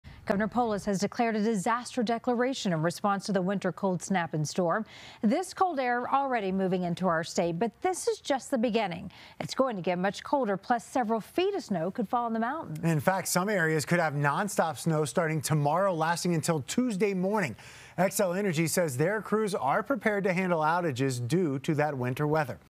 Gov. Polis gives verbal disaster declaration for winter cold snap, storm to open resources
Colorado is expecting a winter cold snap and winter storm, so the state is taking measures to protect people in the state from severe cold.